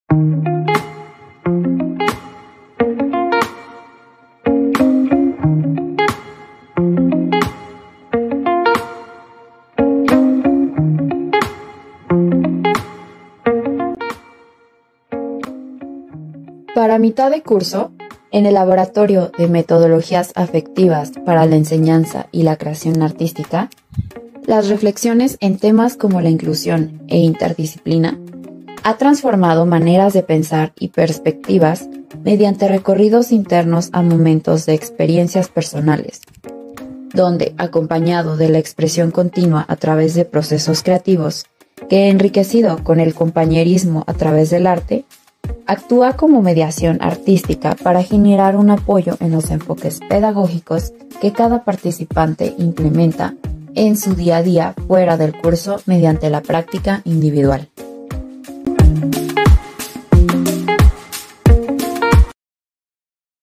OCI | LABORATORIO DE METODOLOGÍAS AFECTIVAS PARA LA ENSEÑANZA Y LA CREACIÓN ARTÍSTICA
Segunda cápsula del curso

Impartieron: Mayela Cardona y Georgina Laorrabaquio

Cápsula realizada por Estefanía Ibarra